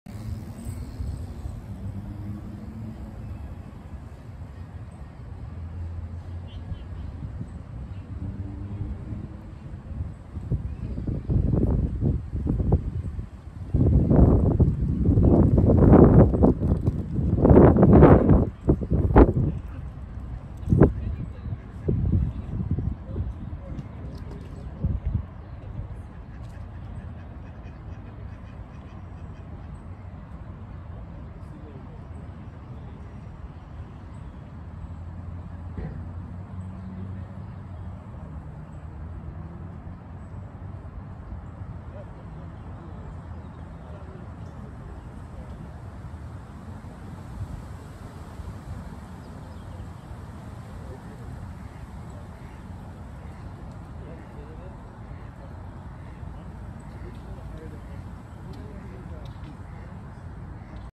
Turtles @ The Palace of Fine Arts Marina San Francisco California
Turtles swim in the lagoon at the Palace of Fine Arts in the Marina District of San Francisco.

4K 30fps